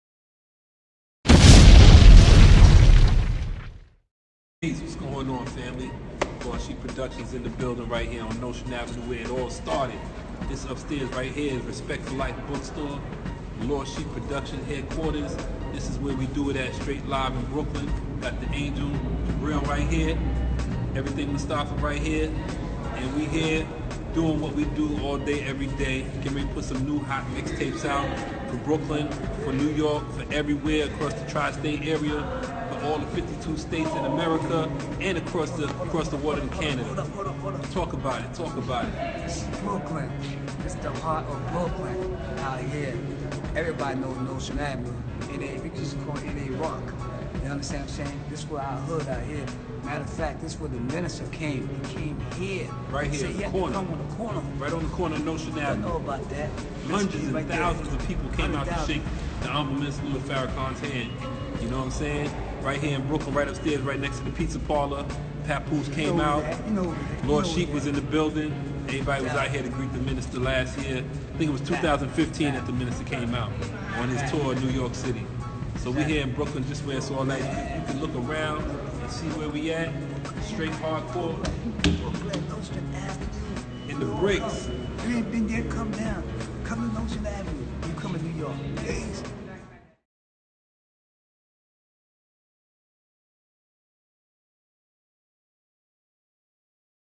Nostrand Ave Shout Out
BROOKLYN!!! What you know 'bout Nostrand Ave.? Watch Bro. Jibriel and Bro. Mustapha give a little history about this legendary street and subway station.